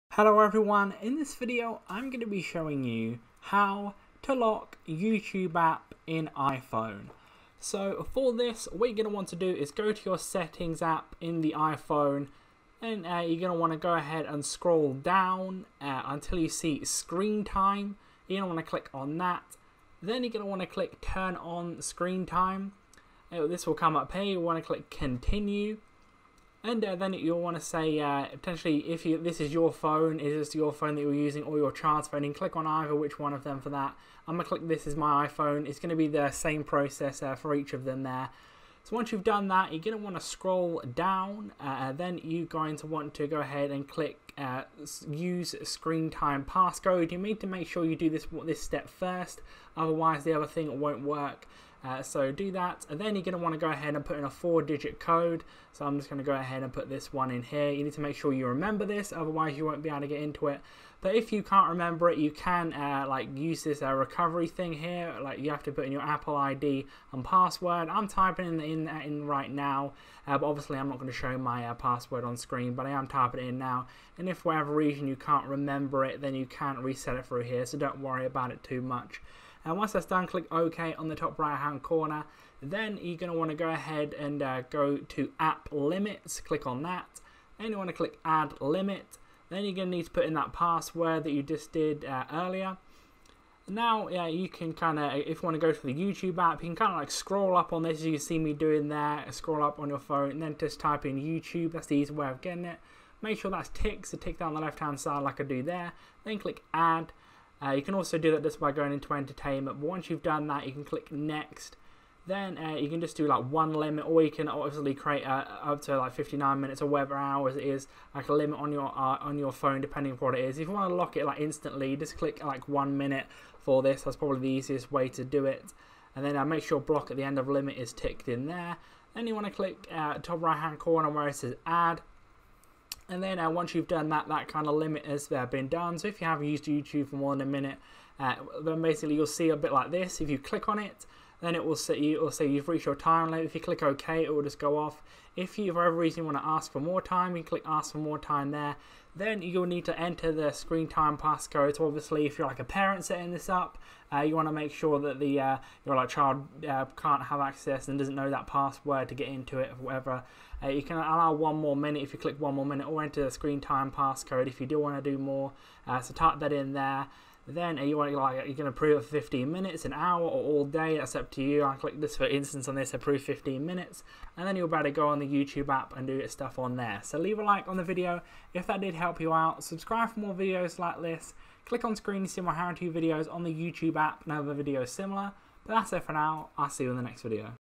How to Lock YouTube App in iPhone or on iPad
How to Lock YouTube App in iPhone or on iPad. Step by step instructions of how to put child lock on youtube app by using screen time limit on your iOS operating systems with the new YouTube app update in 2020. This tutorial guide commentary voice over is done by a native English language speaker.